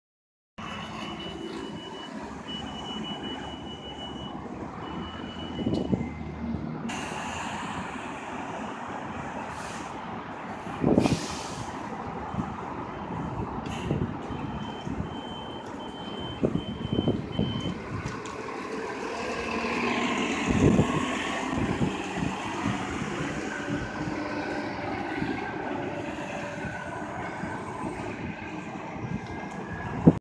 SF Muni 6630, 6633, 6603
Vehicle: 6630, 6633
Route: 38R-Geary Rapid
Model: 2018 New Flyer XDE60
Engine: Cummins ISL9
Transmission: Allison H 50 EP
Division: Flynn

Vehicle: 6603
Route: 38-Geary
Model: 2016 New Flyer XDE60
Engine: Cummins ISL9
Division: Islais Creek

Recorded with my LG Destiny phone.